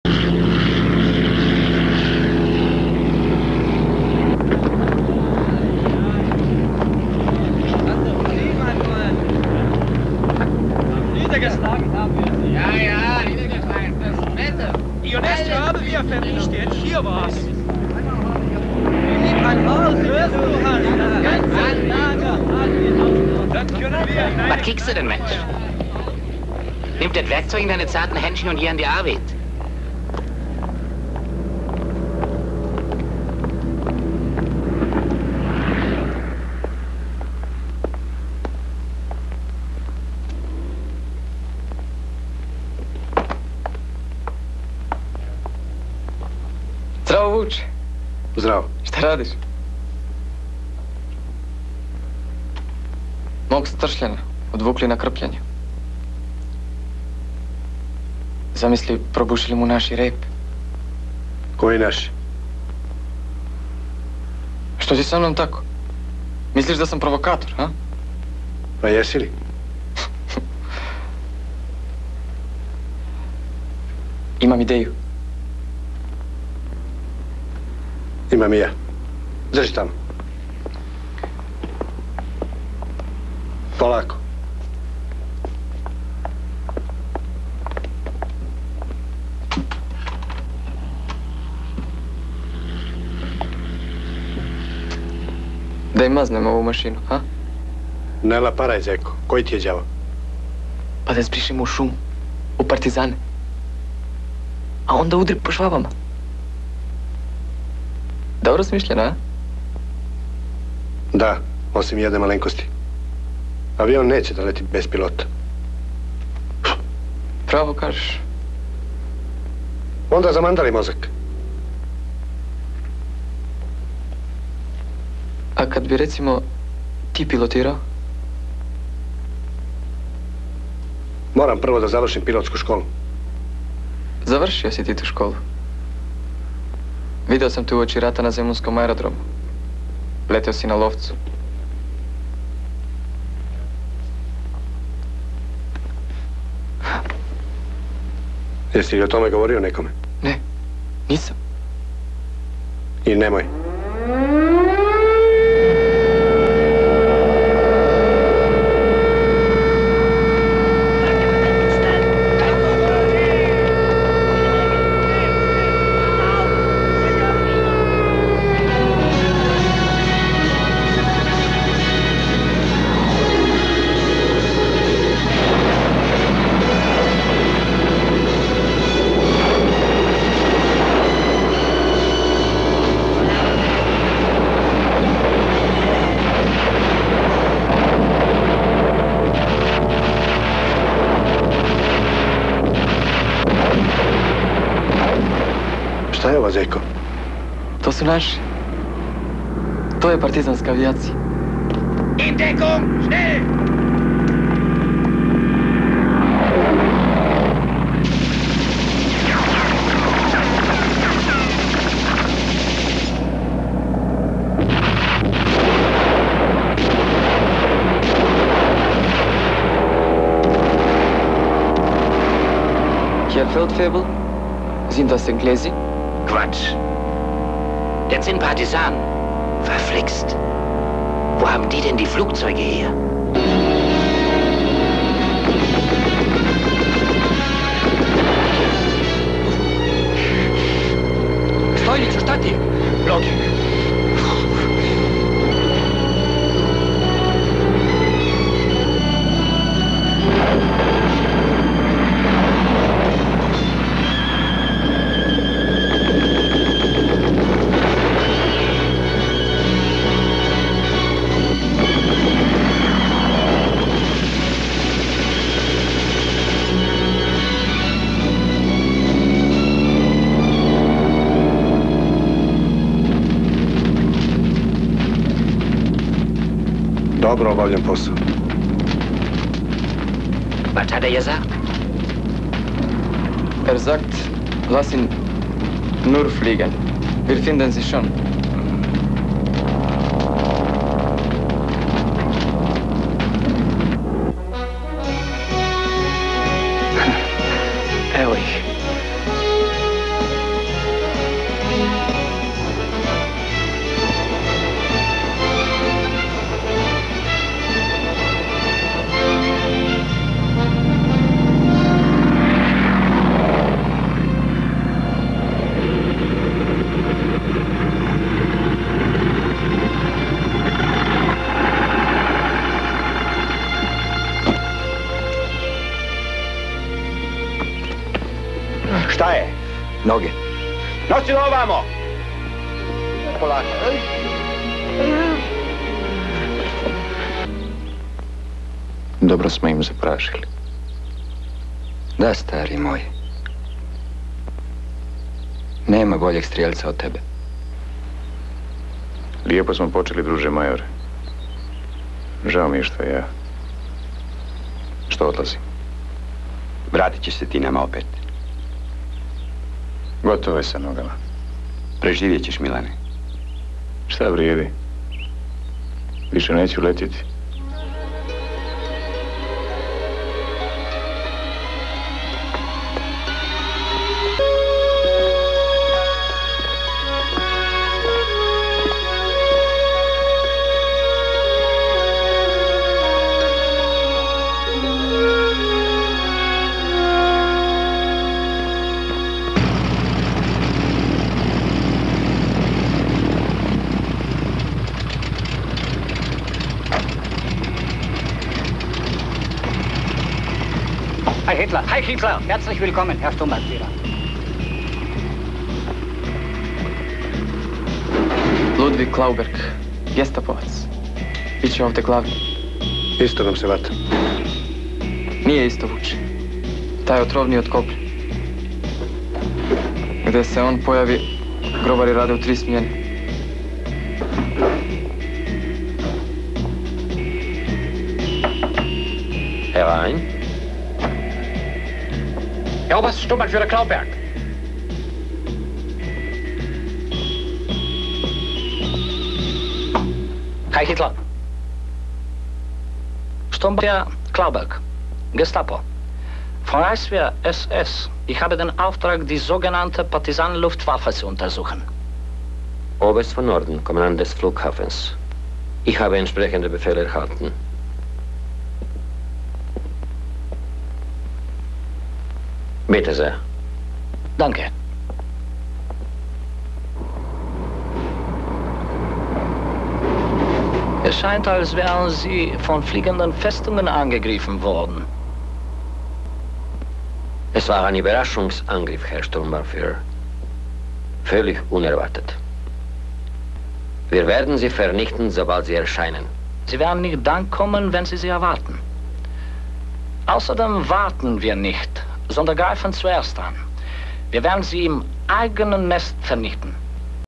Partisanska Espadrila - Luftwafe worst nightmare
Two antique aircraft destroy entire luftwafe airport